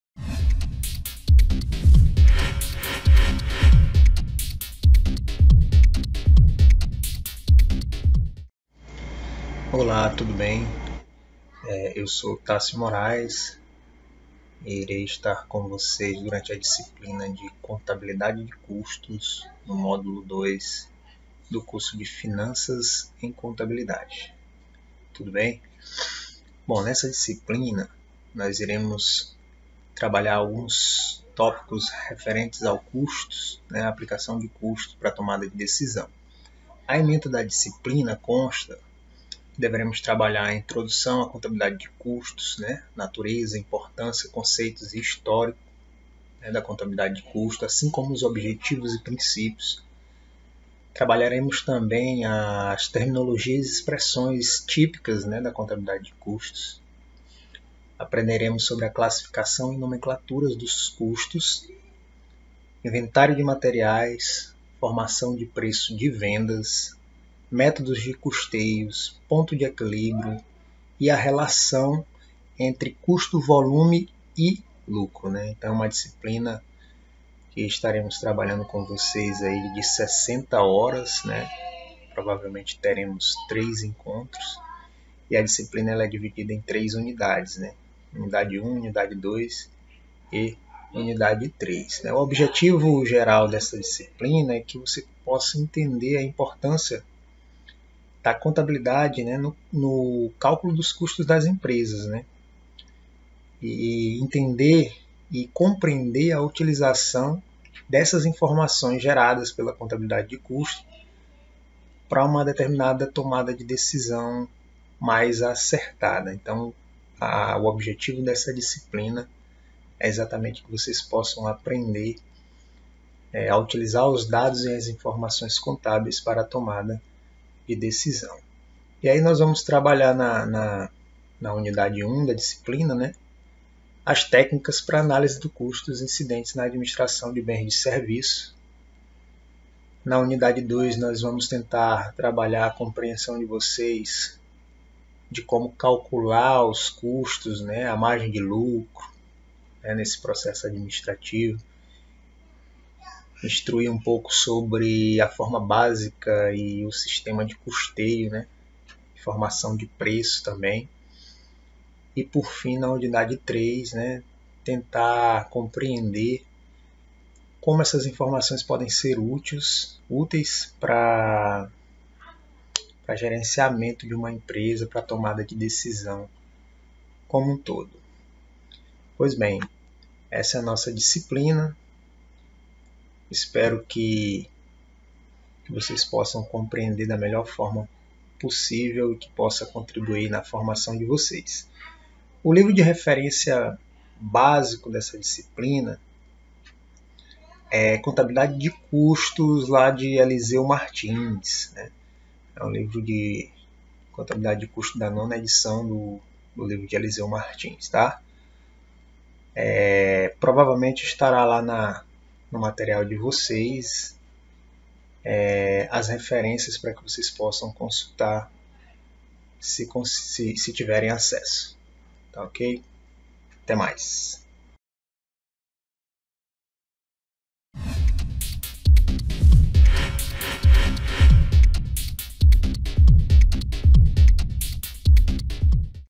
Abertura - Contabilidade de Custos - Prof. Tássio Moraes
Curso: Técnico em Finanças
Disciplina: Contabilidade de Custos